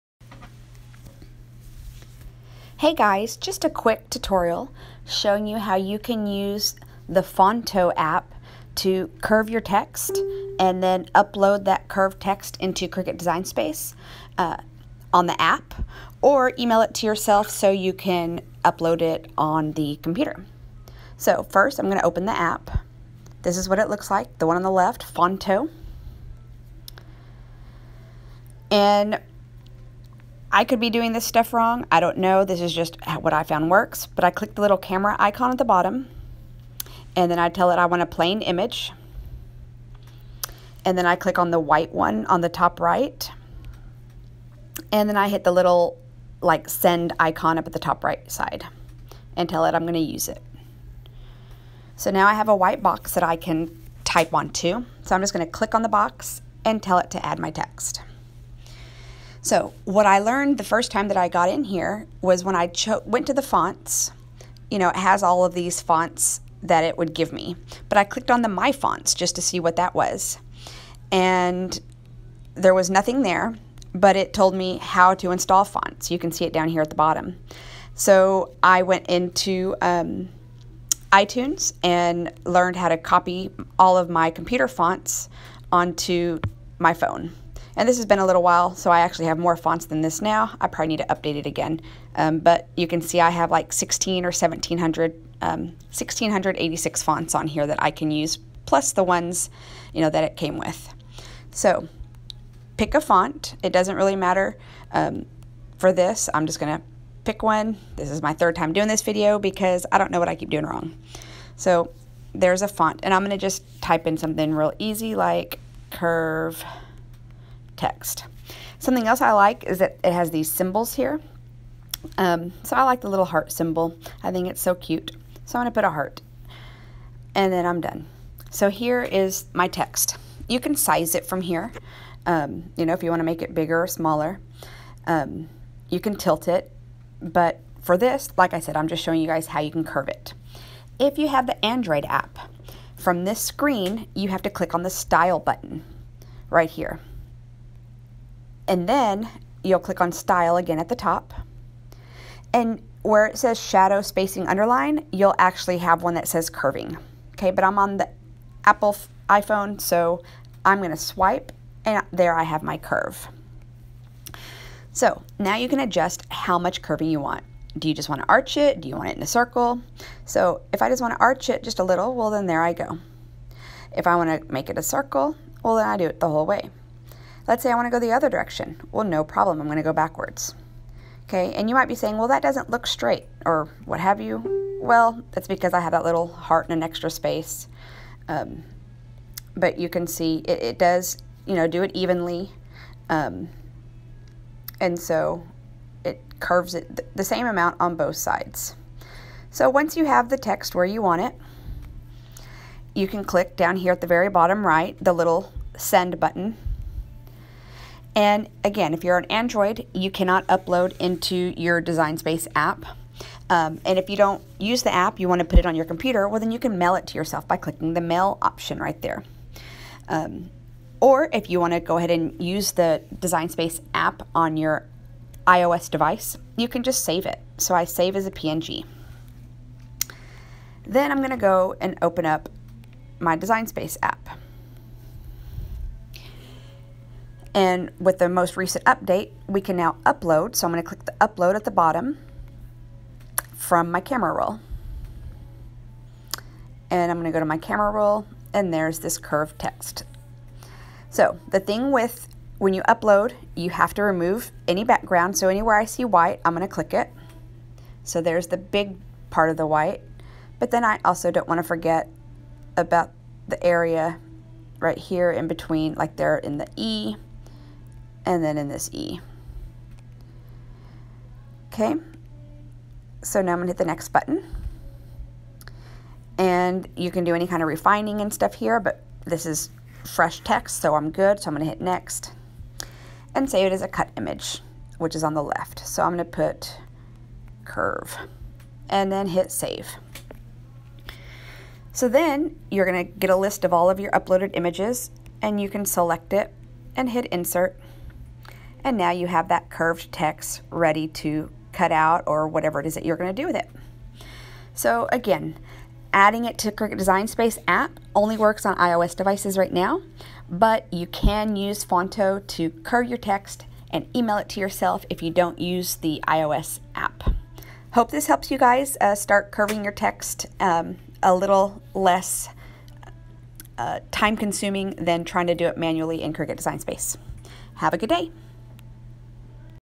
Curving Text using Phonto for Cricut Design Space
Needing to curve your text for your next Cricut project?  Here’s a quick way to curve it using the Phonto app on your mobile device!  Plus, if you have the Cricut Design Space app, you can upload the curved text directly from your iOS device!!  At the time of this video, the CDS Android app doesn’t have that ability.  But you can still send it to yourself to upload on your computer.  Also, the curve feature is in a different spot.  Details given in the video!

If my video helps you, please "like" it so that I know!

I appreciate you watching!!